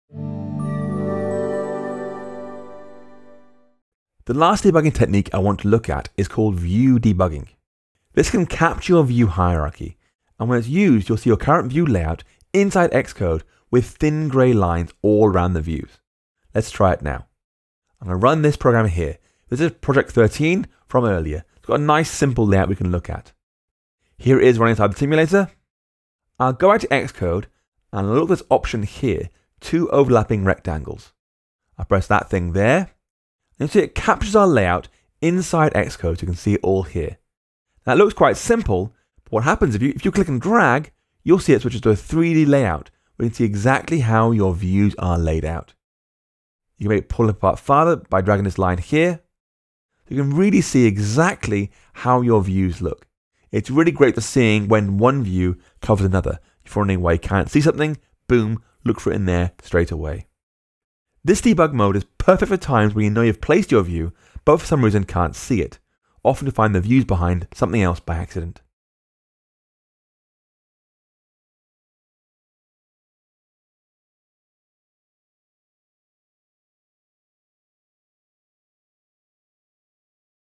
View debugging – Debugging, part 5
In this video we look at a view debugging as a fast and easy way to find hidden views.